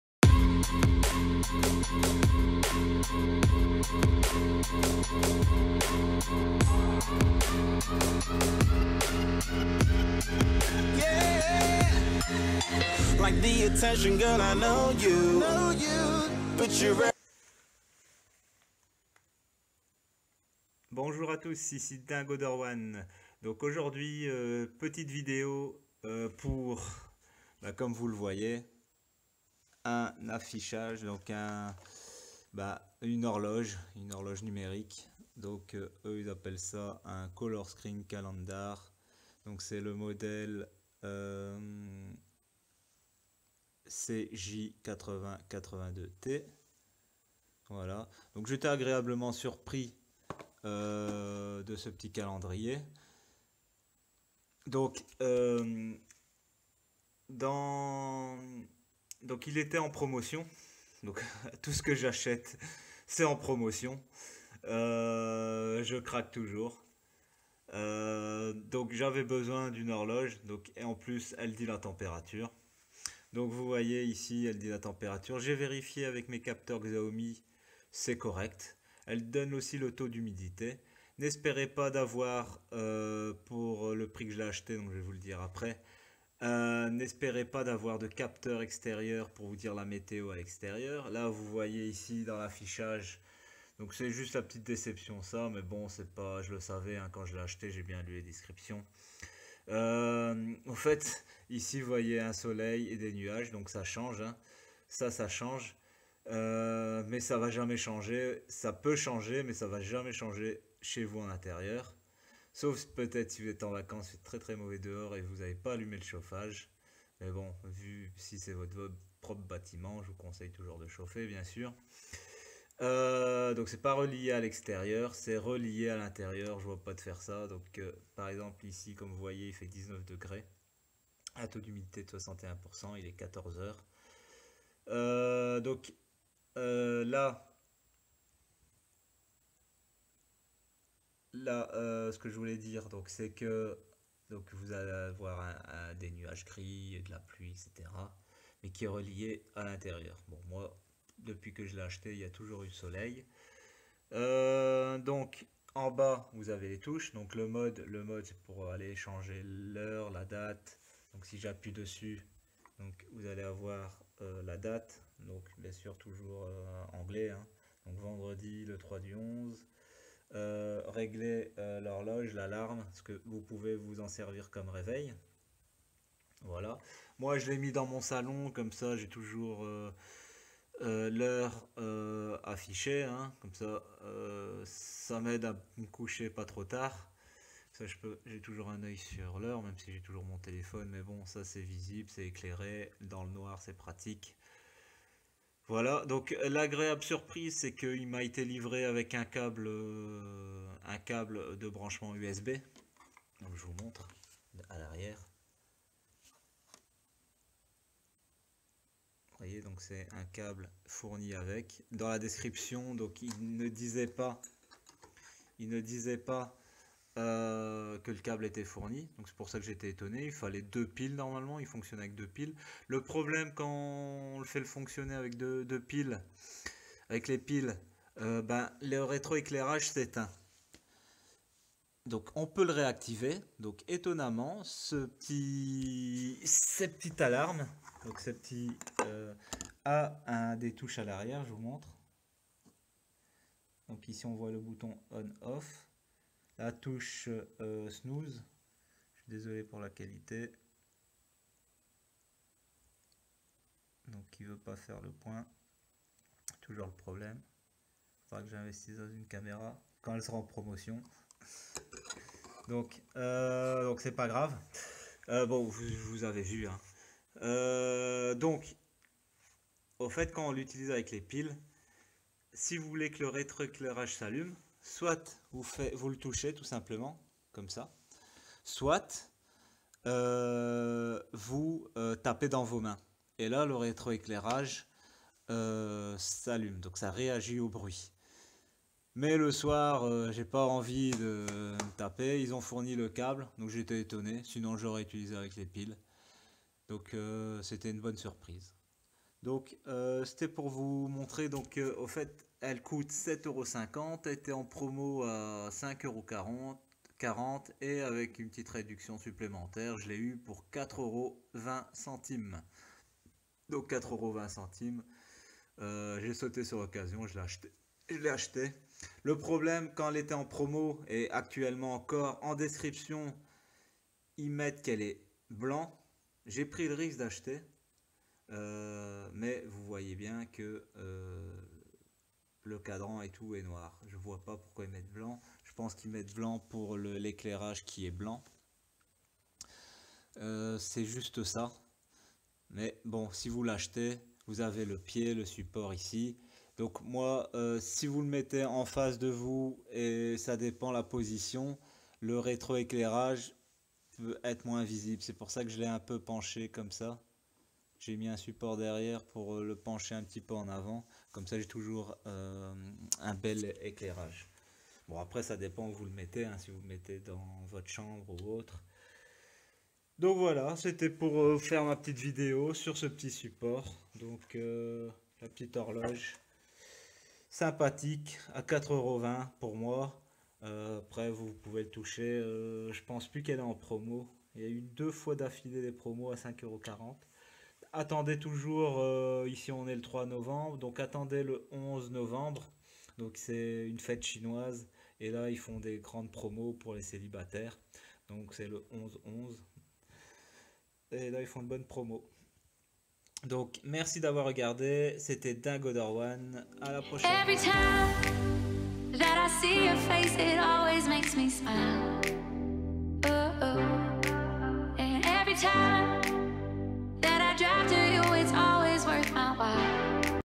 Horloge numérique température, humidité,allumage de l'éclairage au bruit et sensitive
Vidéo de l'horloge numérique avec intro et fin.